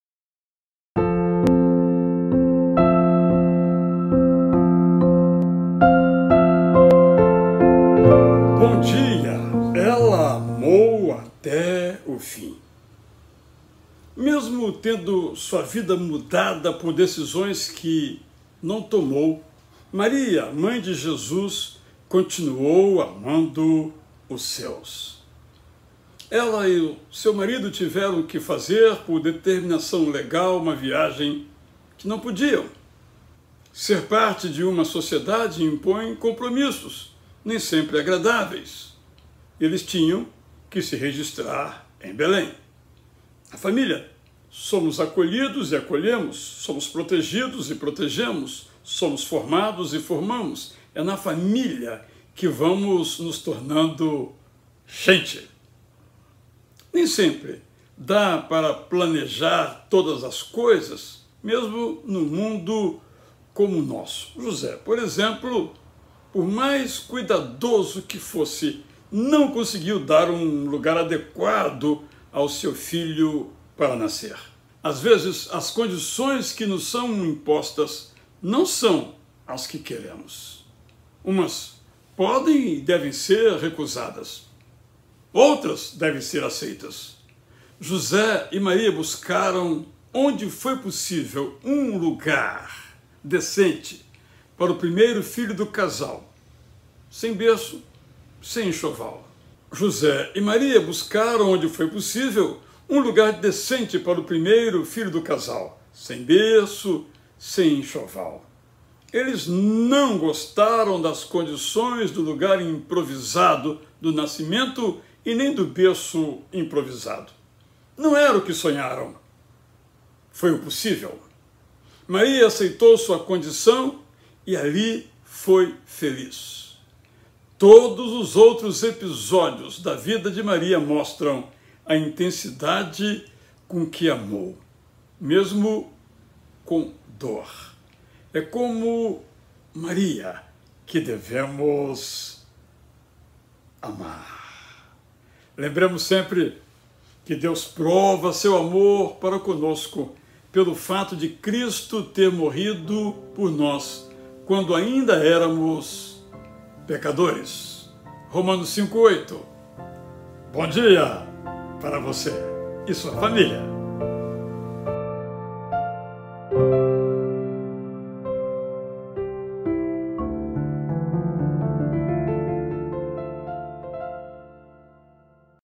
BOM DIA AMIGO, 3229) — Ela amou até o fim
“Não é o casamento que falha; são as pessoas que falham. O que o casamento faz é evidenciar essas falhas”.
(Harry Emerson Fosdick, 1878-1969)

Ela amou até o fim

Mesmo tendo sua vida mudada por decisões que não tomou, Maria, a mãe de Jesus, continuou amando os seus.
Ela e seu marido tiveram que fazer, por determinação legal, uma viagem que não podiam. Ser parte de uma sociedade impõe compromissos, nem sempre agradáveis: tinham que se registrar em Belém.
Na família somos acolhidos e acolhemos. Somos protegidos e protegemos. Somos formados e formamos. É na família que vamos nos tornando gente.
Nem sempre dá para planejar todas as coisas, mesmo num mundo como o nosso. José, por mais cuidadoso que fosse, não conseguiu dar um lugar adequado ao seu filho para nascer.
Às vezes, as condições que nos são dadas não são as que queremos. Umas podem e devem ser recusadas. Outras devem ser aceitas. José e Maria buscaram onde foi possível um lugar decente para o primeiro filho do casal (sem berço, sem enxoval). Eles não gostaram das condições do lugar improvisado do nascimento e nem do berço improvisado. Não era o que sonharam. Foi o possível. Maria aceitou sua condição e ali foi feliz.
Todos os outros episódios da vida de Maria mostram a intensidade com que amou. Mesmo com dor.
É como Maria que devemos amar.

“Deus prova o seu próprio amor para conosco pelo fato de Cristo ter morrido por nós quando ainda éramos pecadores”. (Romanos 5.8)

Bom dia!